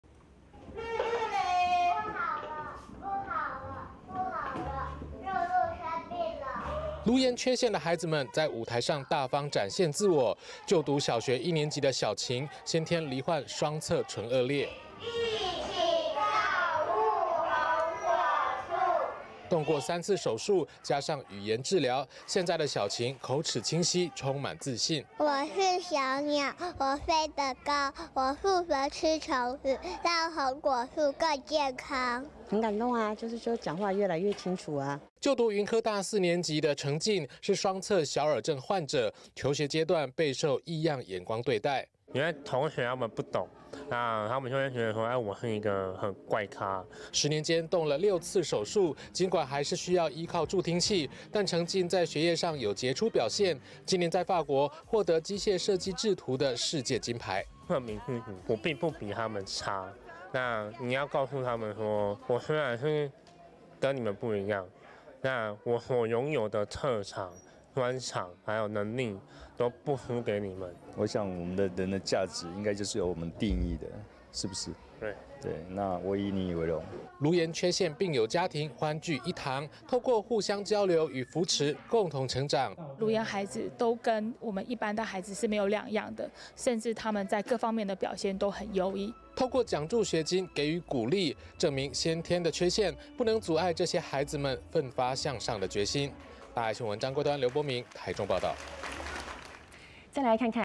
顏損無畏異樣 獎助學金澆灌夢想不流浪
今年就讀大四的張丞瑨，是小耳症患者，但他從不向缺陷低頭，努力向上，在今年國際展能競賽，拿到機械製圖的世界金牌，他的努力，也為所有顱顏缺陷的孩子打了一劑強心針。

顱顏缺陷的孩子們，在舞台上大方展現自我，就讀小學一年級的小晴，先天罹患雙側脣顎裂。動過三次手術，加上語言治療，現在的小晴，口齒清晰，充滿自信。

脣顎裂病友 小晴：「我是小鳥，我飛得高，我負責吃蟲子，讓紅果樹更健康。」

小晴的媽媽：「很感動啊，就是講話越來越清楚。」

就讀雲科大四年級的丞瑨，是雙側小耳症患者，求學階段備受異樣眼光對待。

顱顏青年獎助學金得主 張丞瑨：「因為同學他們不懂，他們就會覺得我是一個怪咖。」

十年間動了六次手術，儘管還是需要依靠助聽器，但丞瑨在學業上有傑出表現，今年在法國獲得機械設計製圖的世界金牌。

顱顏青年獎助學金得主 張丞瑨：「證明自己說，我並不比他們差，你要告訴他們說，我雖然是跟你們不一樣，但我所擁有的特長、專長，還有能力，都不輸給你們。」

顱顏主治醫師 陳潤茺：「我想我們人的價值，應該就是由我們定義的，是不是？(對。)我以你為榮。」

顱顏缺陷病友家庭，歡聚一堂，透過互相交流與扶持，共同成長。

羅慧夫顱顏基金會執行長 陳依伶：「顱顏孩子都跟我們一般的孩子，是沒有兩樣的，甚至他們在各方面的表現都很優異。」

透過獎助學金給予鼓勵，證明先天的缺陷，不能阻礙這些孩子們奮發向上的決心。

張桂端 劉博明 台中報導